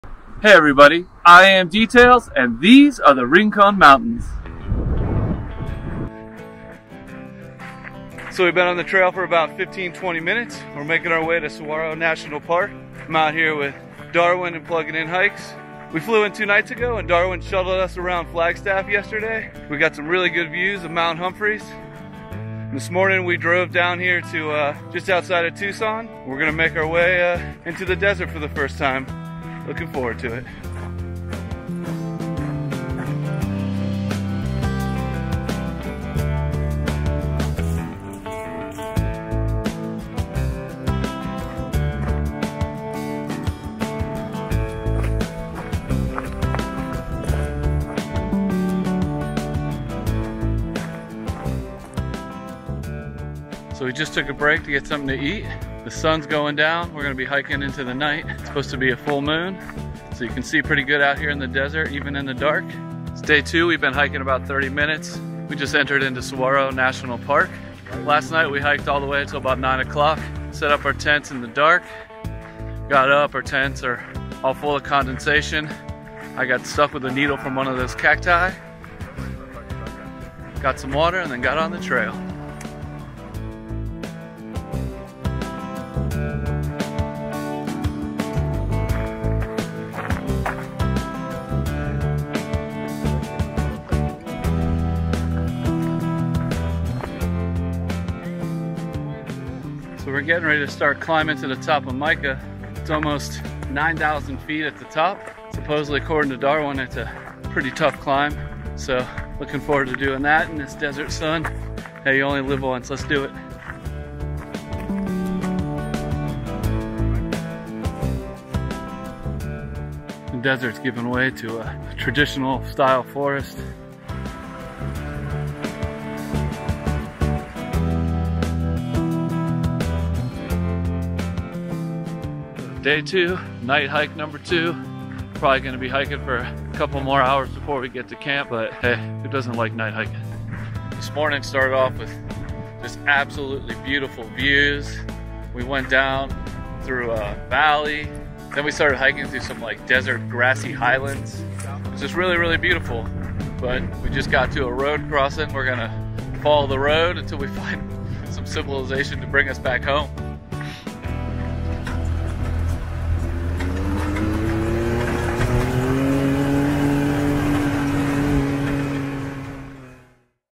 Arizona Trail | Section Hike | Rincon Mountains
My good friend Nic (aka Darwin) invited Benny Braden and myself out to the Arizona for our very first hike in the desert.  This section was full of eco systems foreign to the both of us.  The desert is equal parts brutal and beautiful and its allure will certainly have me back someday soon!